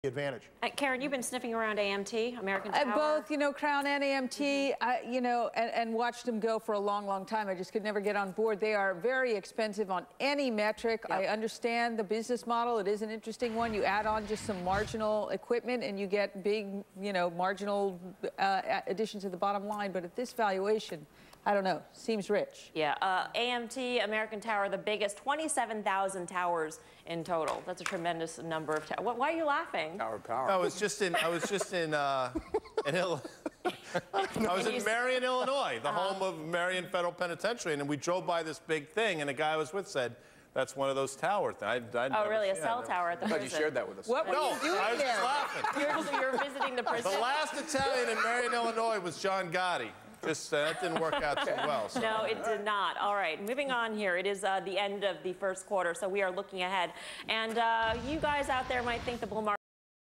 Guy Adami discusses American Tower and mentions Marion IL
Guy Adami  - Marion IL